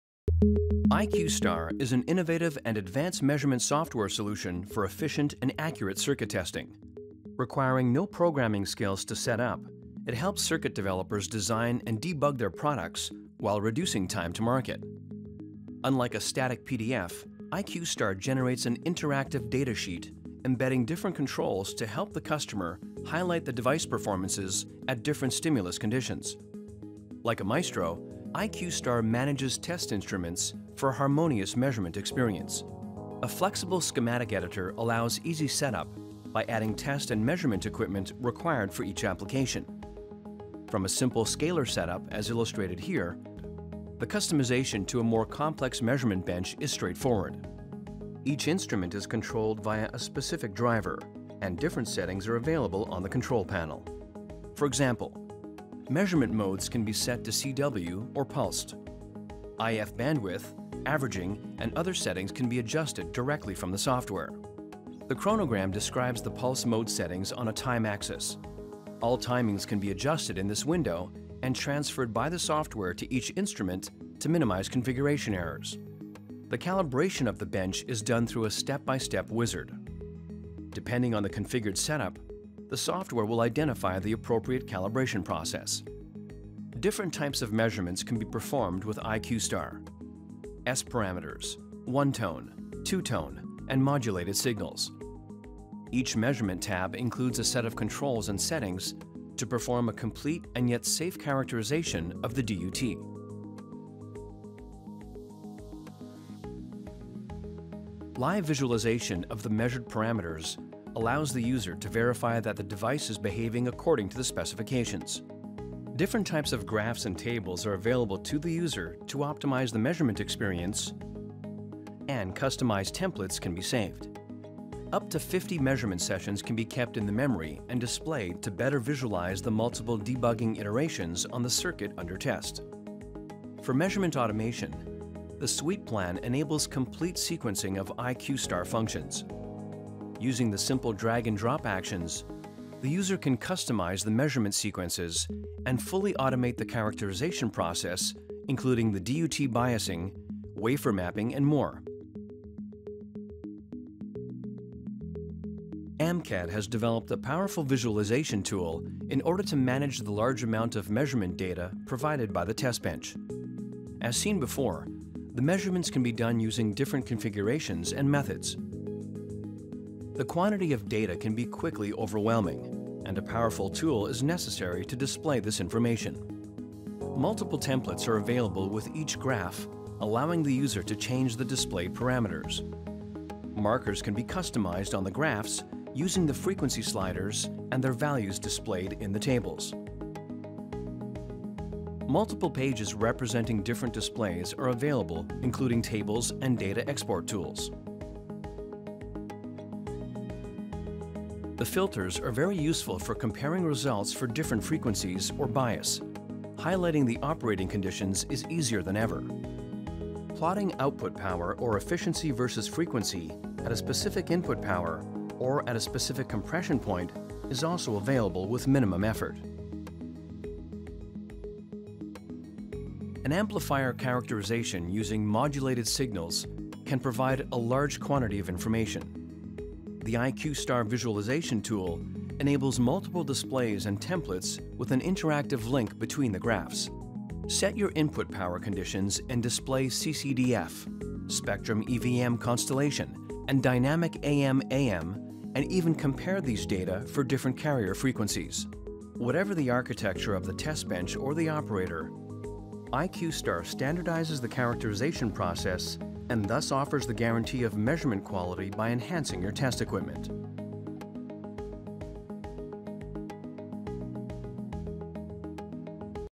IQSTAR
IQSTAR is an innovative and advanced RF & Microwave measurement software solution for efficient and accurate circuit testing.
Requiring no programming skills to set up, it helps circuit developers design and debug their products while reducing time to market.
IQSTAR offers an interactive data management tool, embedding different controls to help the customer highlight the device performances at different stimulus conditions.
Like a Maestro, IQSTAR manages test instruments for a harmonious measurement experience.